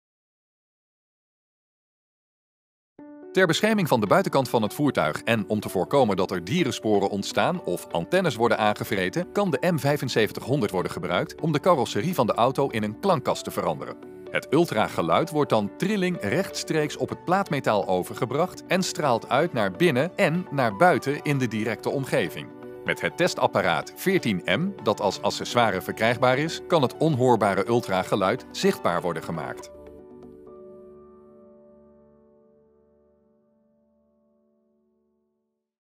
Marterverjaging - M7500: Bescherming binnen en buiten - K&K Marderabwehr
Ter bescherming van de buitenkant van het voertuig en om te voorkomen dat er dierensporen ontstaan of antennes worden aangevreten, kan de M7500 worden gebruikt om de carrosserie van de auto in een klankkast te veranderen. Het ultrageluid wordt dan trilling rechtstreeks op het plaatmetaal overgebracht en straalt uit naar binnen én naar buiten in de directe omgeving. Met het testapparaat 14M, dat als accessoire verkrijgbaar is, kan het onhoorbare ultrageluid zichtbaar worden gemaakt.